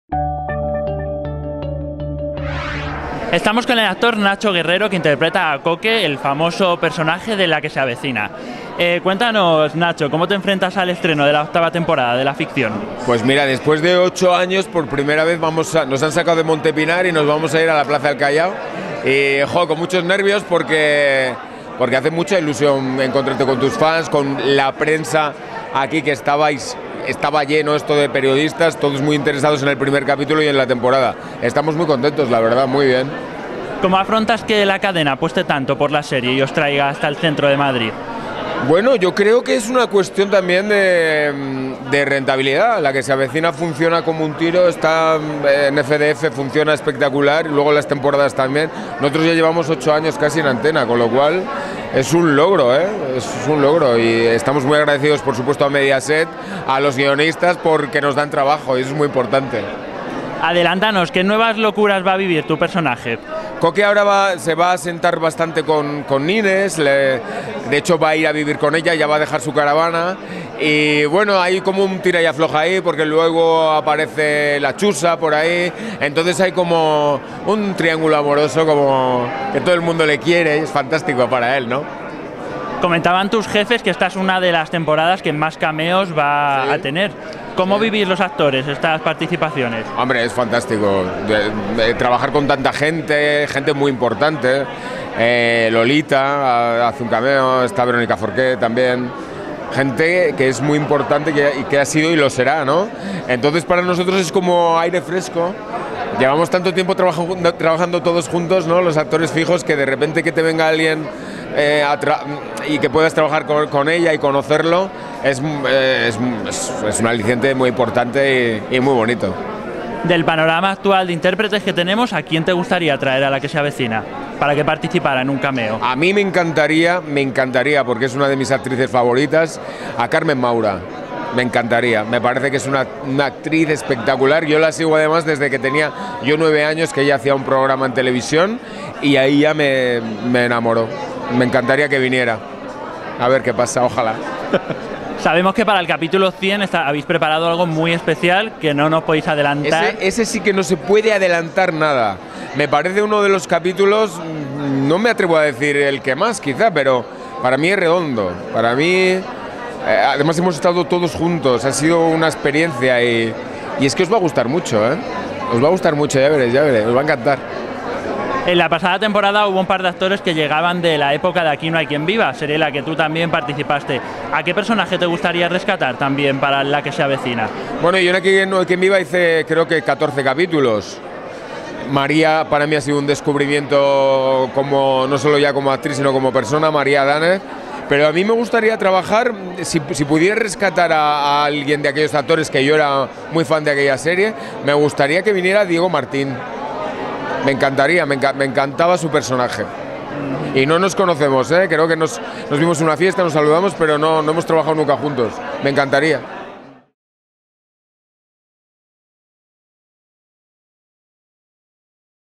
Nacho Guerreros ('La que se avecina'): "Coque vivirá un triangulo amoroso en la nueva temporada"
Con motivo del estreno de la nueva temporada de la comedia de Telecinco, el actor Nacho Guerreros nos cuenta sus impresiones sobre las nuevas tramas de la serie: "Os va a gustar mucho el capítulo 100".